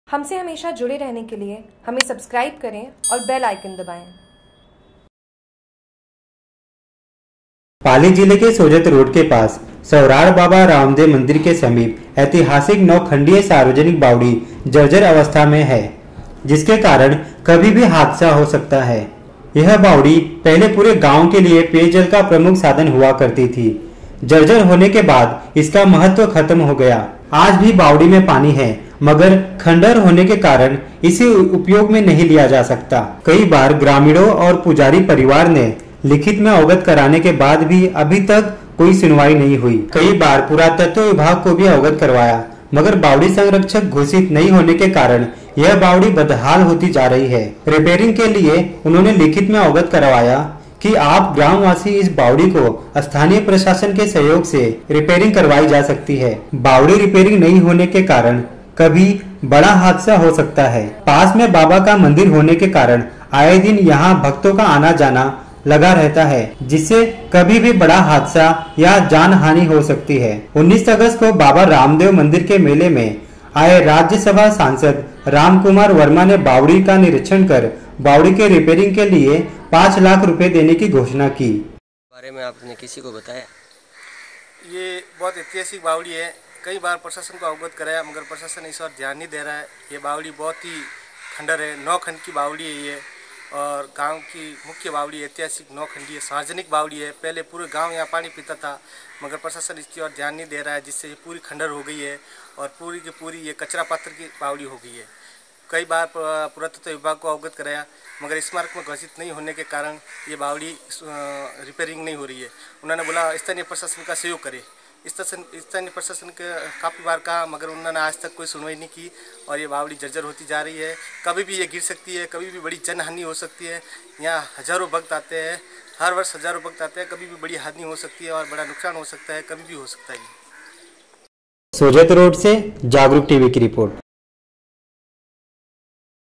JAGRUK TV : सोजत रोड : जर्जर हाल में ऐतिहासिक बावड़ी
पाली जिले के सोजत रोड के पास सवराड़ बाबा रामदेव मंदिर के समीप ऐतेहासिक नो खंडीय सार्वजनिक बाबडी जो जर्जर अवस्था में पडी होने के कारण कभी भी हो सकता है हादसा प्रसाशन नहीं दे रहा है इस और ध्यान यह बावड़ी पहले पुरे गांव के लिए पेय जल का प्रमुख साधन हुआ करती थी जो जर्जर होने के बाद इसका महत्त्व ख़त्म को गया आज भी बावड़ी में पानी है मगर खंडहर होने के कारण नहीं आ रहा पानी उपयोग। कई बार ग्रामीणों ने व् पूजारी परिवार में लिखीत में अवगत कराने के बाद भी अभी तक नहीं हुइ सुनवाई  कई बार पुरातत्व विभाग को भी अवगत करवाया मगर बावड़ी सरक्षक घोषित नहीं होने के कारण यह बावड़ी पुरातत्व विभाग ऐसे नहीं करा सकता है रैपिरिंग के लिए उन्होंने लिखित में अवगत करवाया की आप ग्राम वासी इस बावड़ी को स्थानीय प्रशाशन के सहयोग से बावड़ी की रैपिरिंग कार्रवाई जा सकती है